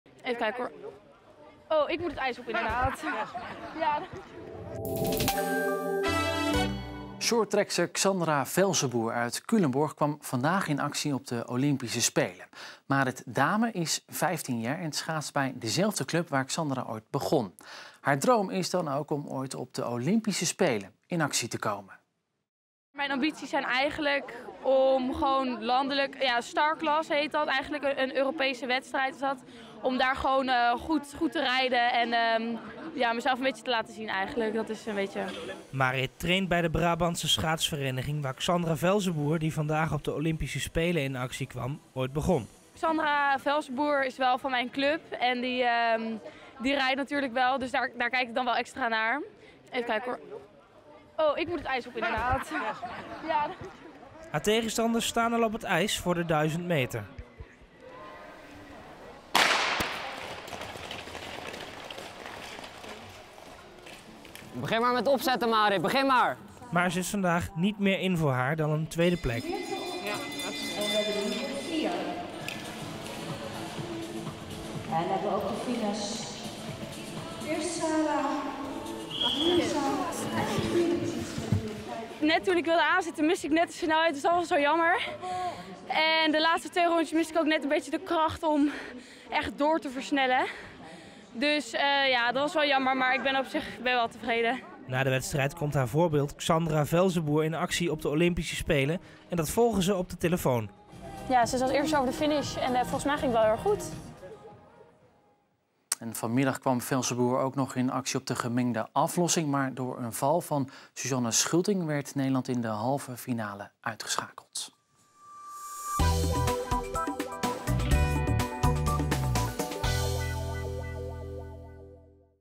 Marit wil net als Xandra Velzeboer naar de Olympische Spelen
Shorttrackster Xandra Velzeboer uit Culemborg komt uit op de Olympische Spelen. Iets waar ploeggenoot Marit Damen ook van droomt.